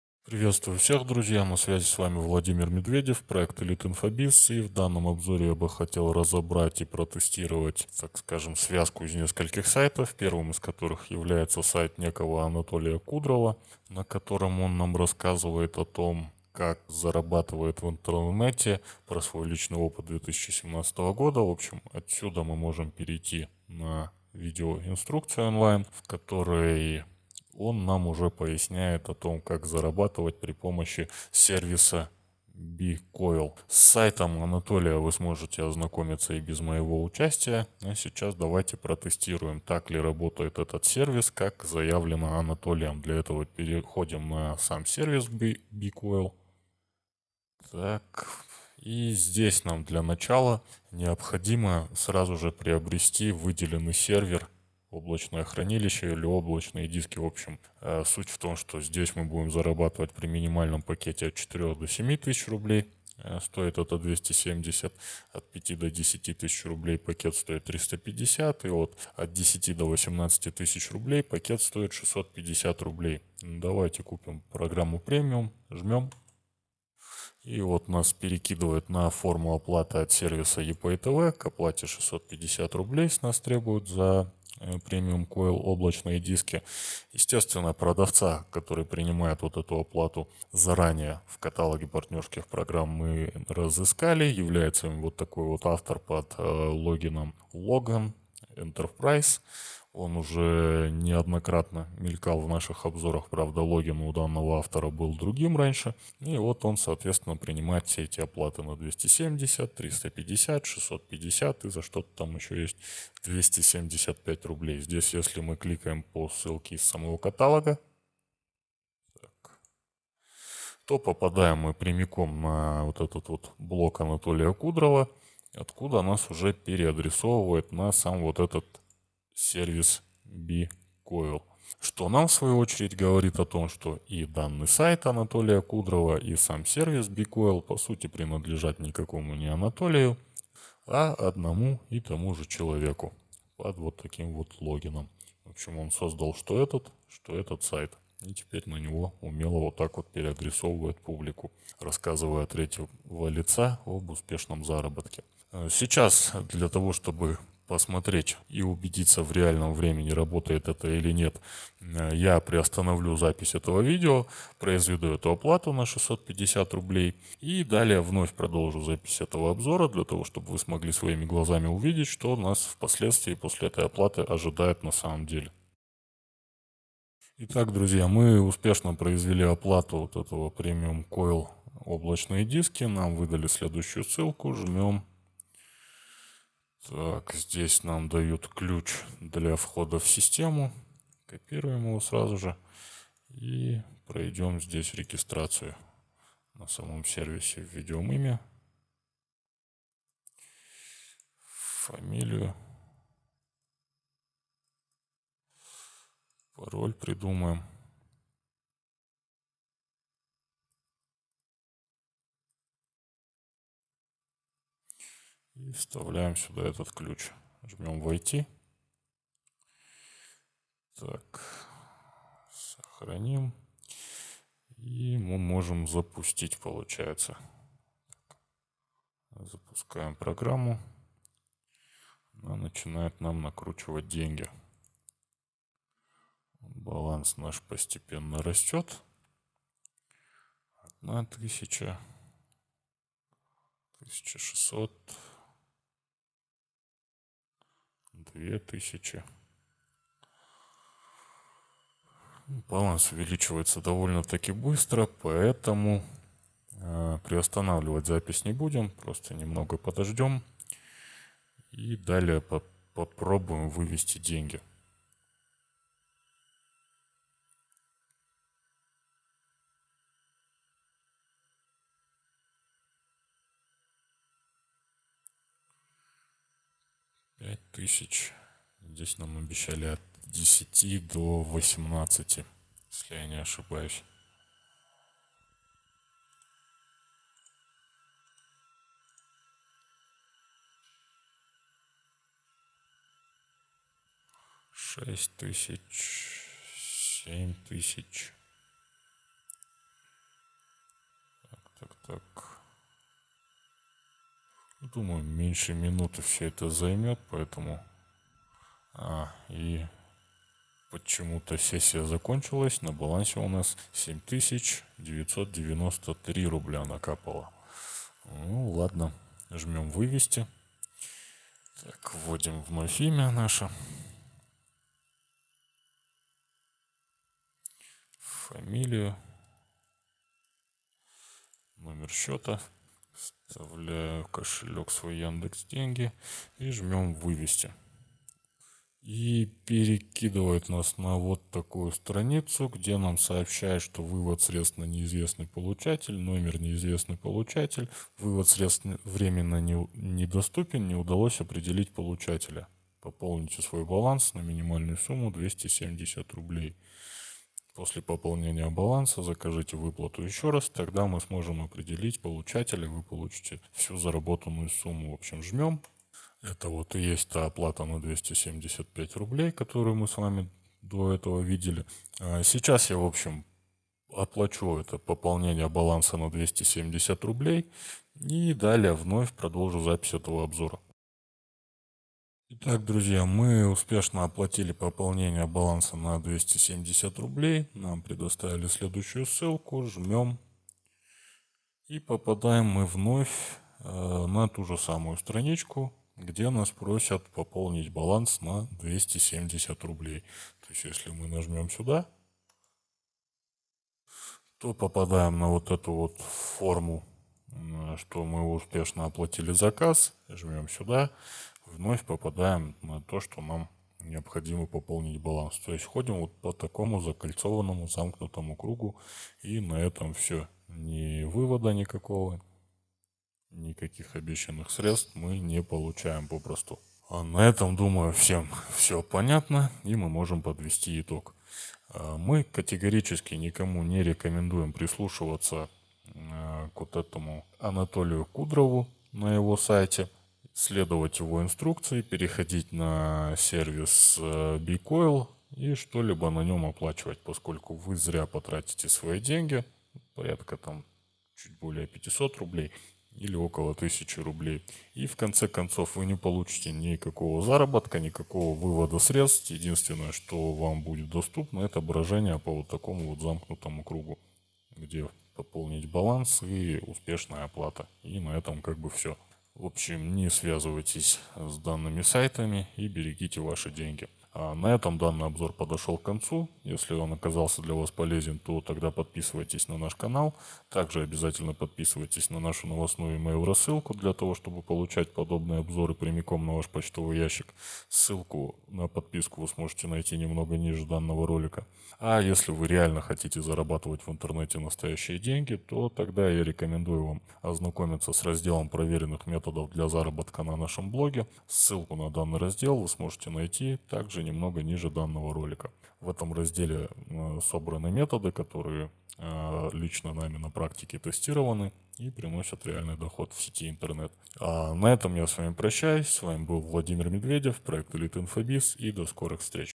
Анатолий Кудров и липовый обзор сервиса B-KOIL. Честный отзыв.
Анатолий Кудров на своем сайте произвел обзор сервиса B-KOIL в деталях. В прямом эфире показывая, как он зарабатывает и выводит из сервиса B-KOIL более 10000 рублей. Здесь же вам перенаправляют на сайт сервиса где собственно происходит "заработок". Смотрите наш обзор и не попадайтесь на этот лохотрон!

Если Вам надоело платить мошенникам и Вы хотите получать из сети интернет реальную прибыль, то ознакомьтесь с нашим разделом проверенных методов для заработка